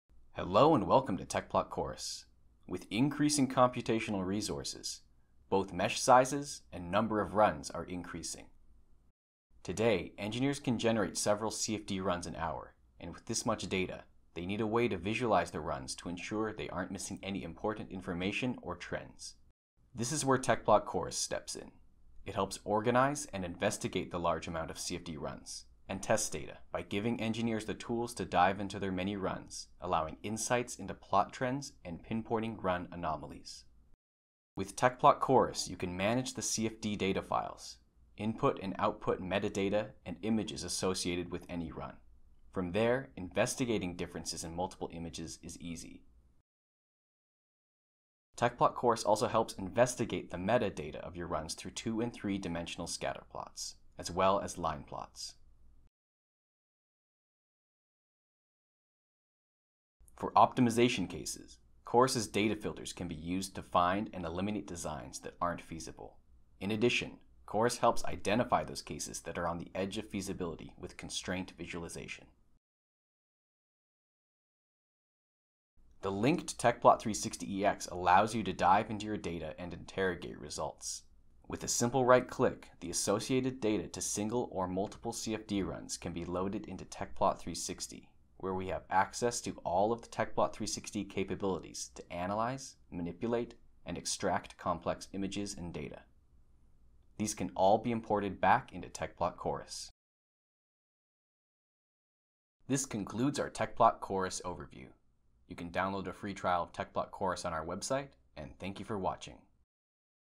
Tecplot Chorus Overview
This video show you the capabilities of Tecplot Chorus.

With increasing computational resources, both mesh sizes and number of runs are increasing. Today, engineers can generate several CFD runs an hour and with this much data they need a way to visualize the runs to ensure they aren’t missing any important information or trends.

This is where Tecplot Chorus steps in. It helps organize and investigate the large amount of CFD runs and test data by giving engineers the tools to dive into their many runs allowing insights into plot trends and pin pointing run anomalies.